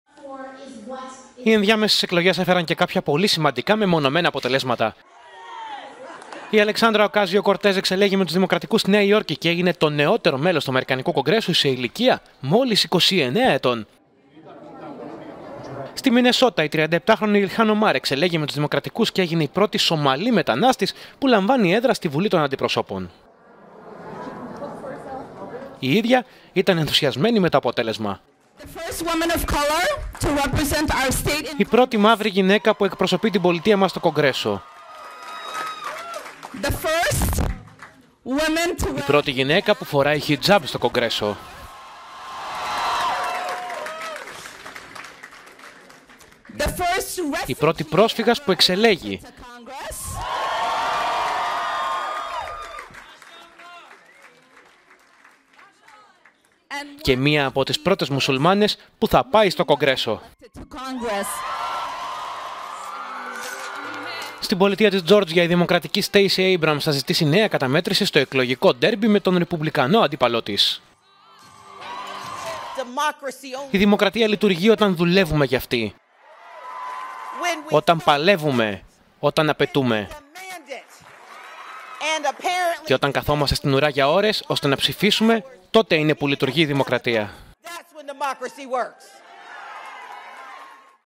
Εκλογές ΗΠΑ: Τα αποτελέσματα που ξεχώρισαν
Μία 29χρονη και μία Σομαλή πρόσφυγας στο Κογκρέσο των ΗΠΑ…